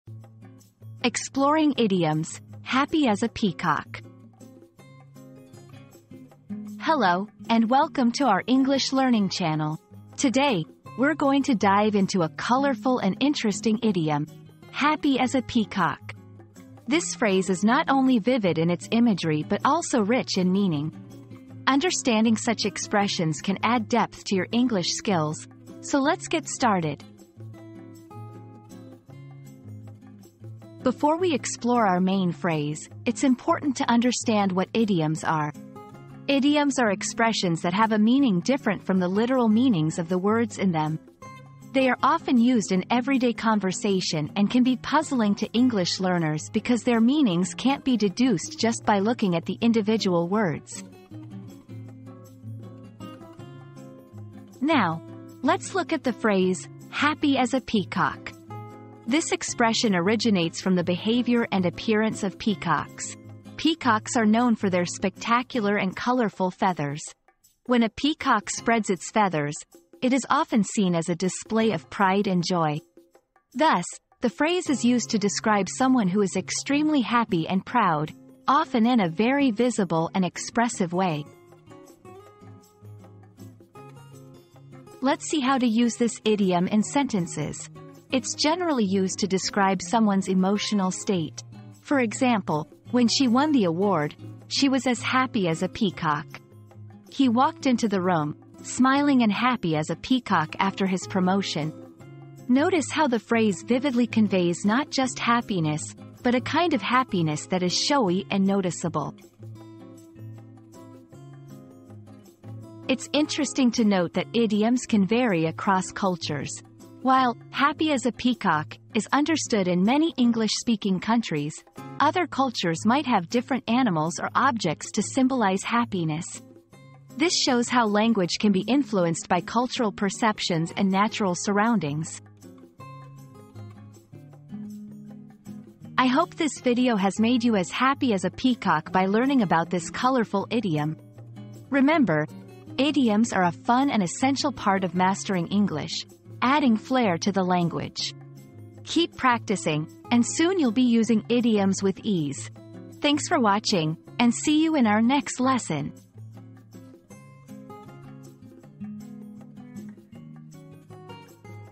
Exploring Idioms: "Happy as a Peacock"
Exploring Idioms: Happy as a Peacock • Uncover the fascinating origins and meaning behind the idiom 'Happy as a Peacock' in this engaging exploration. Discover the surprising history and symbolism associated with this colorful expression that captures the essence of pure joy and contentment.

00:00 • Introduction - Exploring Idioms: "Happy as a Peacock"
00:30 • What is an Idiom?
00:54 • Origins of "Happy as a Peacock"
01:24 • Using "Happy as a Peacock" in Sentences
01:52 • Cultural Context and Variations